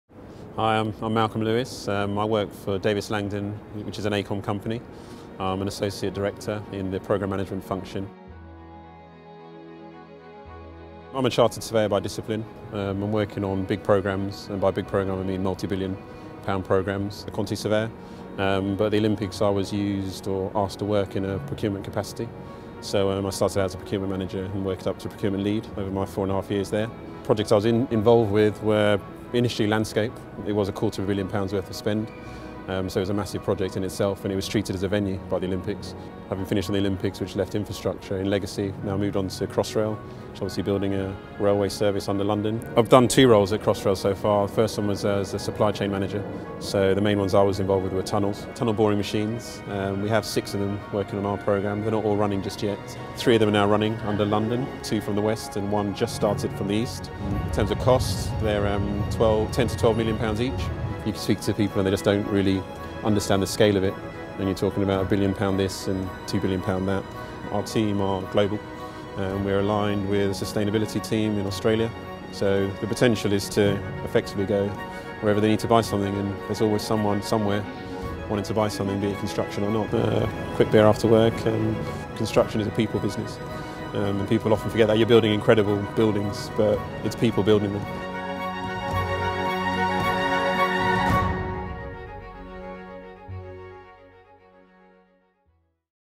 Two minutes with quantity surveyor Malcolm Lewis
In our latest video, we spend time with quantity surveyor Malcolm Lewis as he takes us through his typical day as Crossrail procurement assurance manager. Based at Canary Wharf in London, Malcolm has a leading role in delivering the process for minimising supply chain failure for the major Crossrail construction projects with a combined value in excess of £1bn (mainly on tunnel, shaft and portal works).